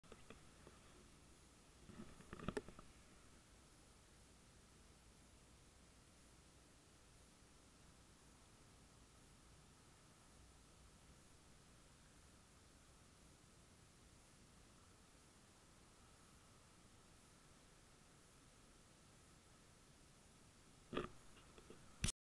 Kamera sportowa Soocoo S60 - test pod słońce
Nagranie zarejestrowane nową kamerką sportową Soocoo S60 - plik wrzucony prosto z karty pamięci, bez dodatkowej obróbki.